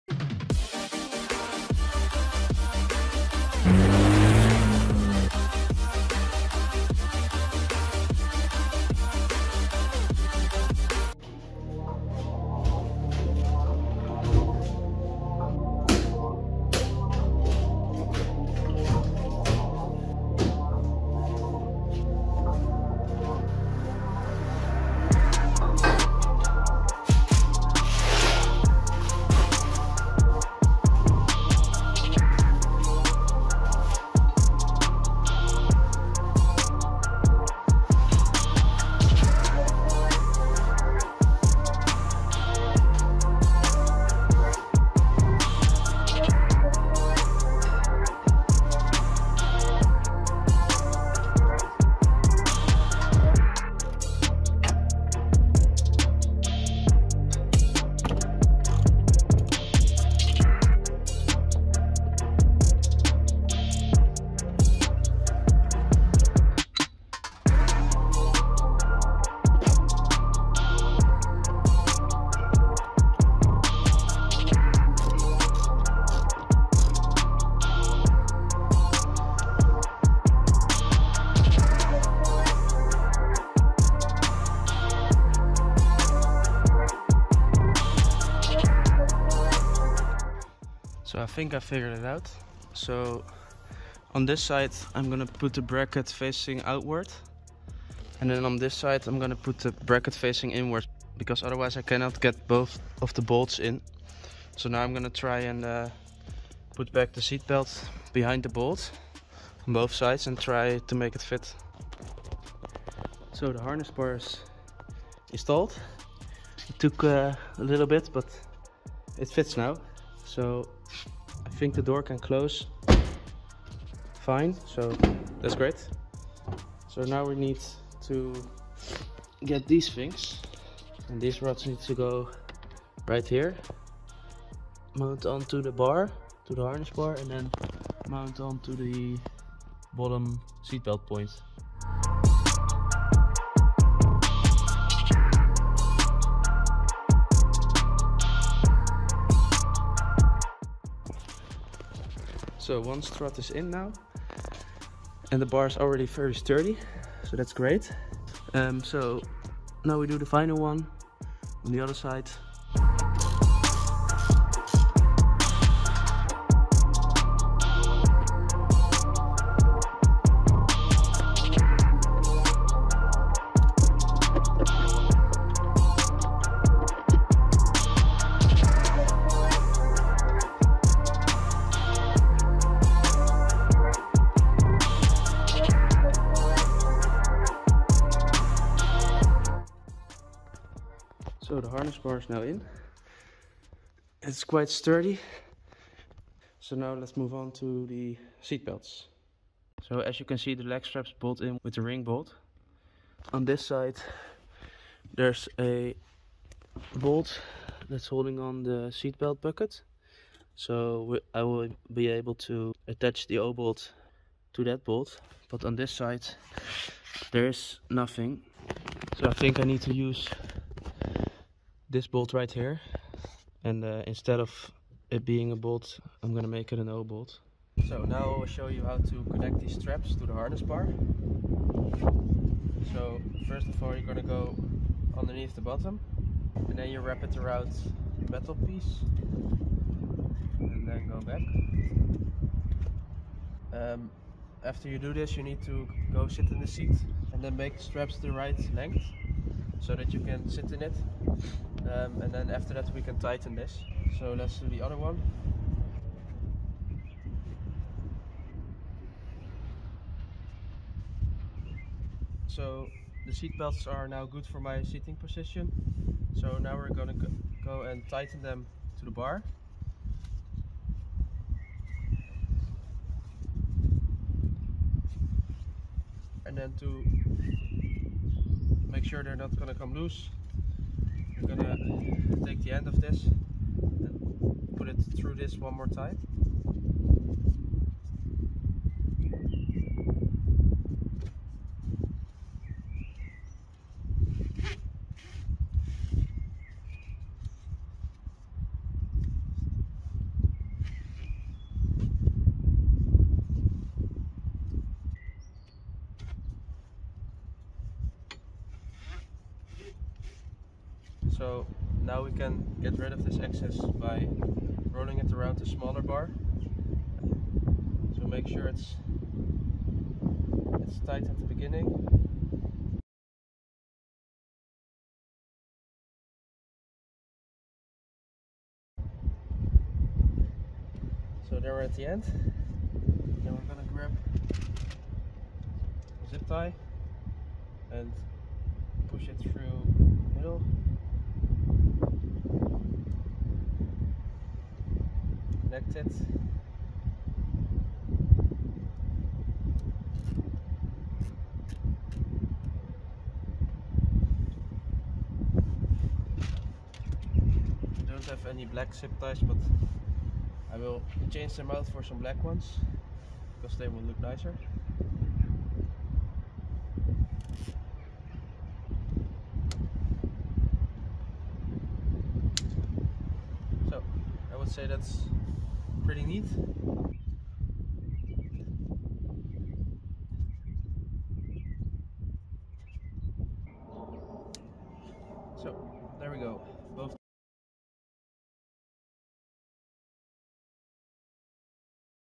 Universal Harness Bar | 200HP Peugeot 206 2.0 hdi
In this video, I'll show you how to install a harness bar on my Peugeot 206. A harness bar is an important safety feature for car enthusiasts who want to take their vehicle to the track or participate in motorsport events.

I'll walk you through the simple and easy process of installing the harness bar, including where to position the bar and how to secure it properly. You won't need any specialized tools for this installation, just basic hand tools that most car owners have.

So, whether you're a seasoned car enthusiast or a beginner looking to upgrade your vehicle's safety features, this video will provide you with all the information you need to install a harness bar on your Peugeot 206.